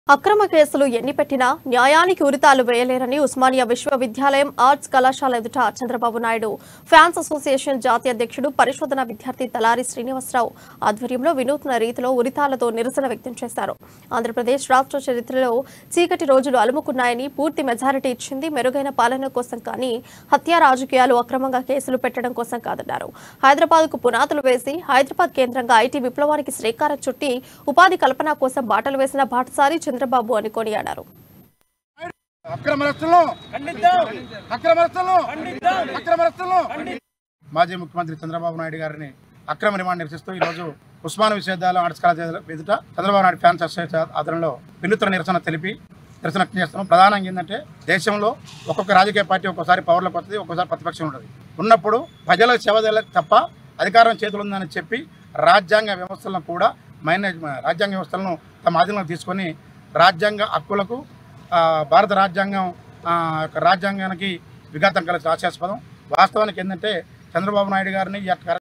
Osmania Students Protest For Chandrababu | ఉస్మానియా ఎదుట ఉరితాళ్లతో విద్యార్థుల నిరసన | Amma News
Chandra Babu | Osmania University | ఉస్మానియా ఎదుట ఉరితాళ్లతో విద్యార్థుల నిరసన | Amma News